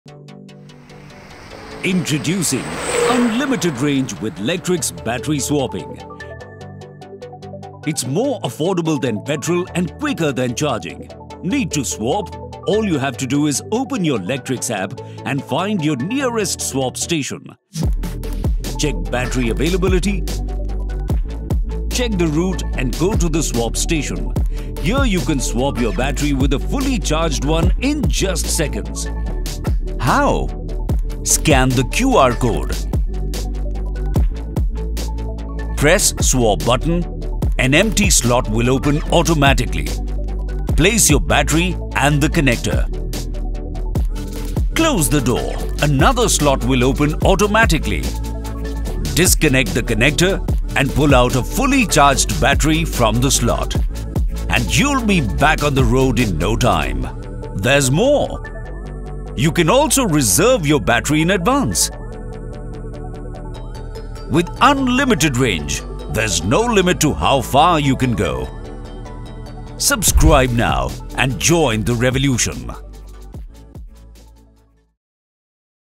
LECTRIX EV | END-TO-END SWAP SOLUTION
In a world where time is money and sustainability is key, Lectrix EV presents the ultimate solution: a battery swap model that's fast, affordable and most importantly reliable.

Swapping your EV battery has never been easier! With Lectrix EV, simply:

•⁠  ⁠Open the Lectrix app
•⁠  ⁠Reserve a fully charged battery at your nearest swap station
•⁠  ⁠Drive up to the station
•⁠  ⁠Let our automated system swap your battery in 52 seconds
•⁠  ⁠Drive away with unlimited range

No lengthy process, no waiting, no fuss. It's that simple!